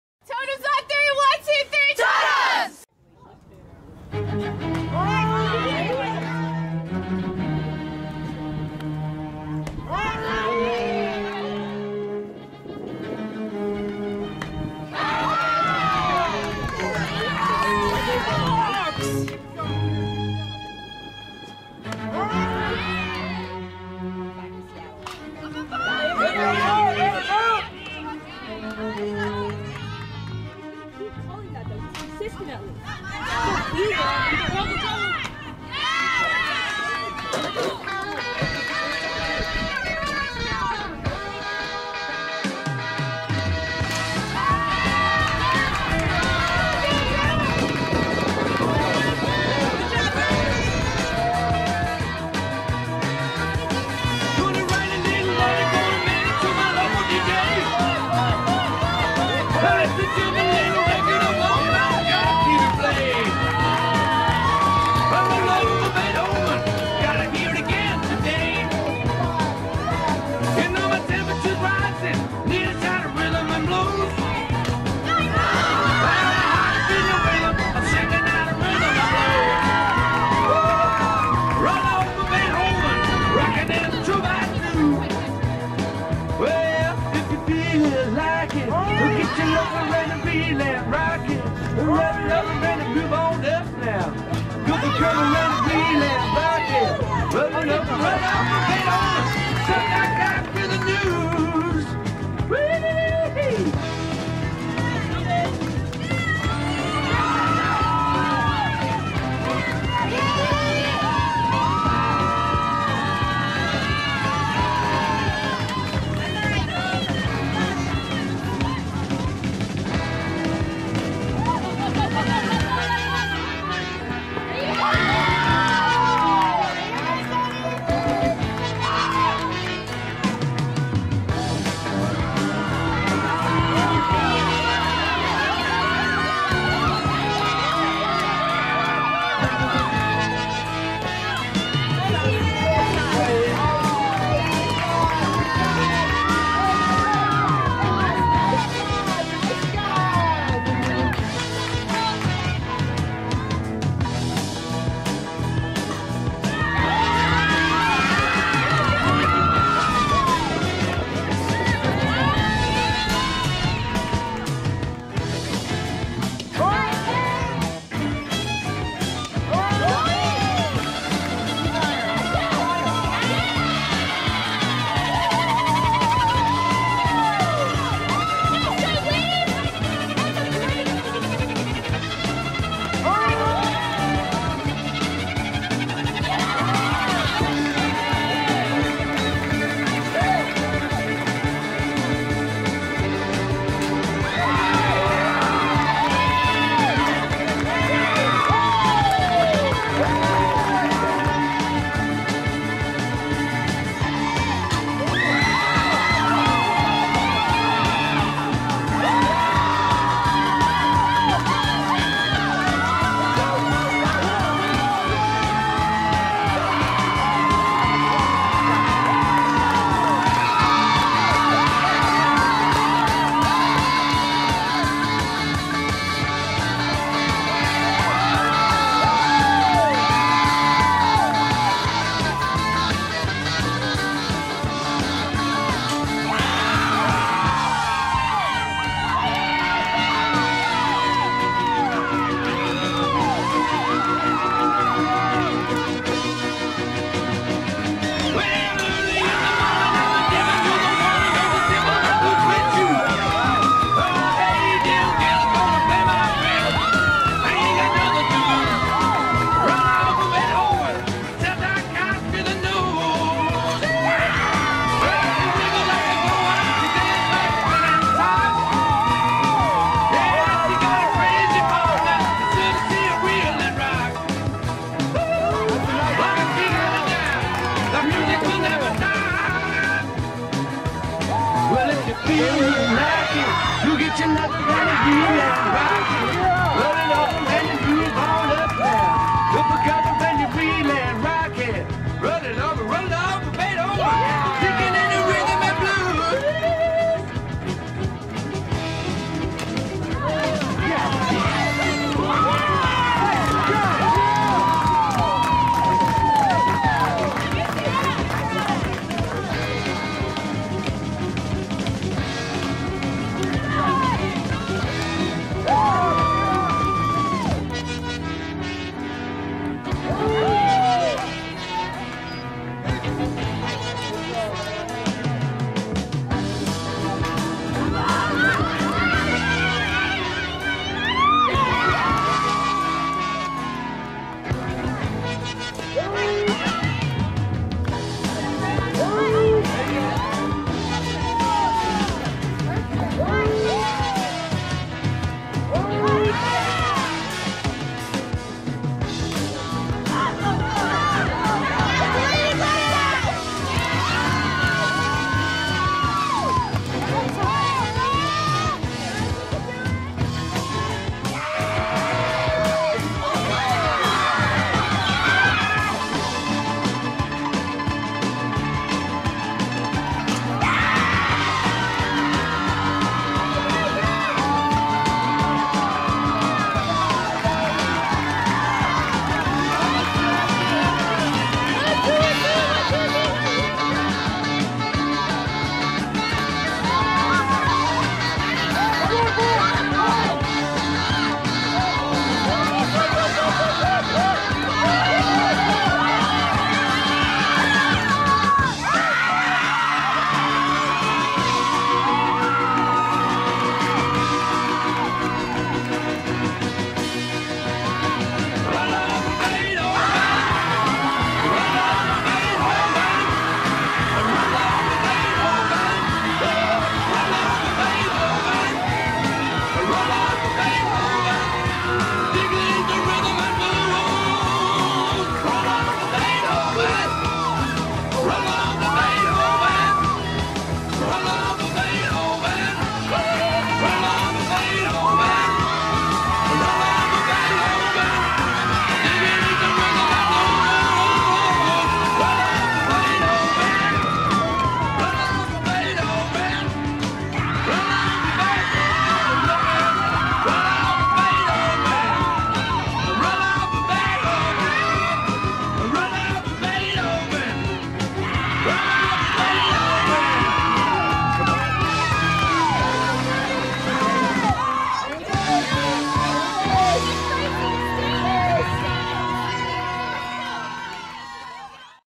2023 SHS Varsity Fastpitch vs Chief Sealth G2 - HIGHLIGHTS with music
March 27th at Chief Sealth
Second game of a double-header.